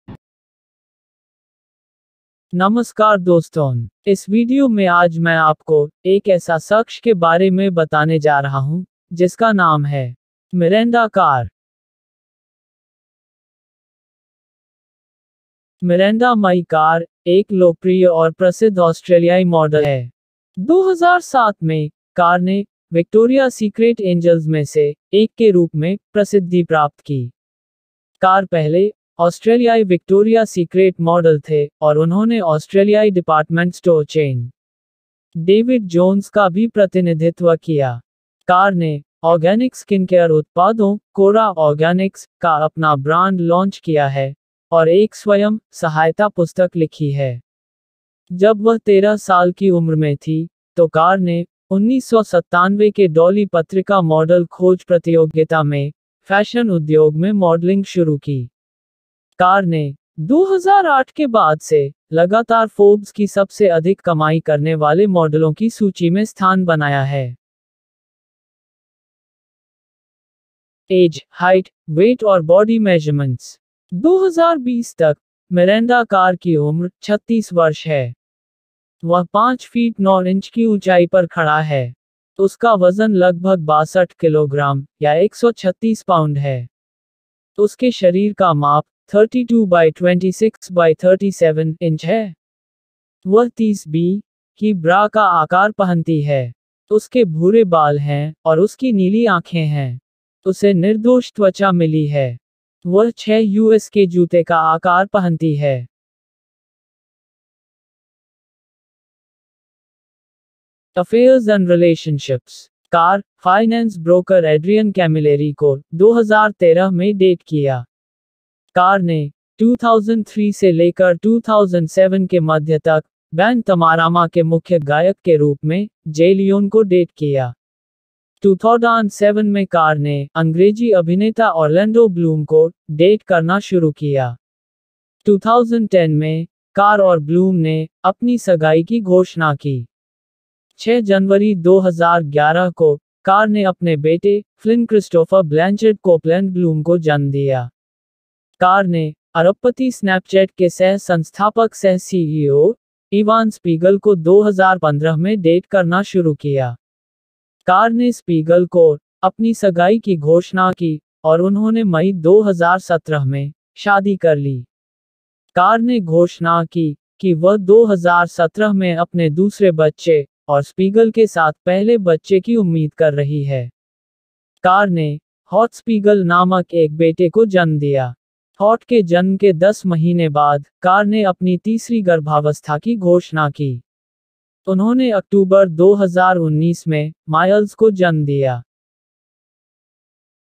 Miranda Kerr | Australian Model, Businesswoman, Victoria's Secret Angel |Biography, Lifestyle
Hello Friends ...
In this video, I am going to share full details about an Australian model, businesswoman and Victoria's Secret Angel "Miranda Kerr"

•• Hollywood Models & Celebs :-